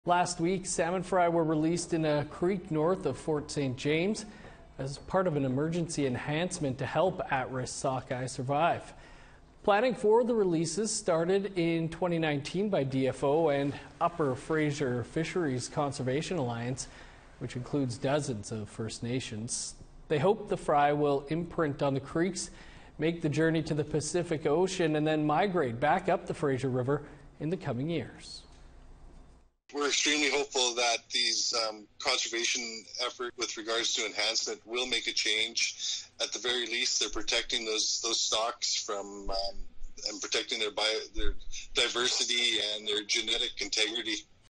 Over 60,000 sockeye fry released to support salmon populations in B.C. | APTN News
B.C. First Nations along with Fisheries and Oceans Canada released over 60,000 sockeye fry to support salmon populations impacted by the Big Bar landslide.

They hope the fry will imprint on the creeks and migrate back up the Fraser River in the coming years.